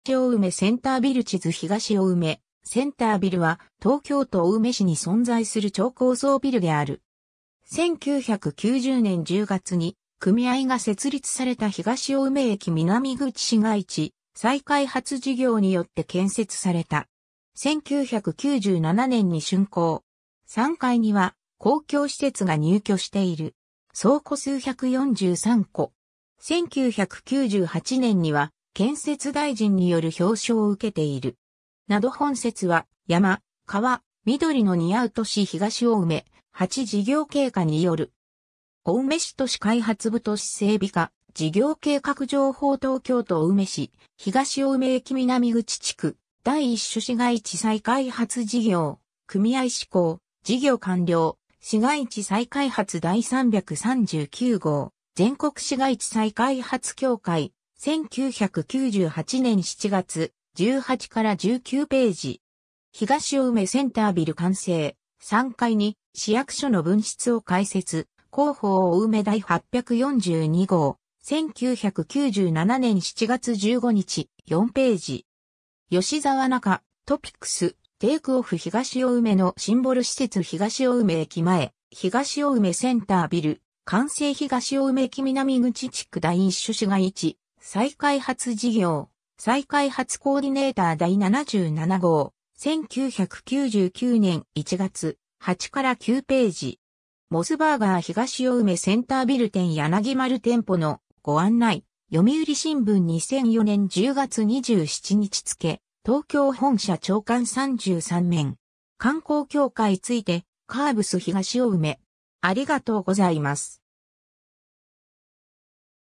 東青梅センタービル
東青梅駅と東青梅センタービル 地図 東青梅センタービル（ひがしおうめセンタービル）は、東京都青梅市に存在する超高層ビルである。
 1990年（平成2年）10月に組合が設立された東青梅駅南口市街地再開発事業によって建設された。
1997年（平成9年）に竣工。
3階には公共施設が入居している。
総戸数143戸。
1998年には建設大臣による表彰を受けている。
 など 本節は『山・川・緑の似合う都市東青梅』「8 事業経過」による。
 ^ 青梅市都市開発部都市整備課「事業計画情報 東京都青梅市・東青梅駅南口地区（建物名称: 東青梅センタービル）―第一種市街地再開発事業・組合施行/事業完了―」『市街地再開発』第339号、全国市街地再開発協会、1998年7月、18-19ページ。
 ^ 「東青梅センタービル完成、3階に市役所の分室を開設」『広報おうめ』第842号、1997年7月15日、4ページ。
 ^ 吉澤中「トピックス 「テイクオフ東青梅」のシンボル施設 東青梅駅前 東青梅センタービル 完成 東青梅駅南口地区 第一種市街地再開発事業」『再開発コーディネーター』第77号、1999年1月、8-9ページ。
 ^ モスバーガー東青梅センタービル店 ^ 柳丸 店舗のご案内 ^ 『読売新聞』2004年（平成16年）10月27日付東京本社朝刊33面。
 ^ 観光協会ついて ^ カーブス東青梅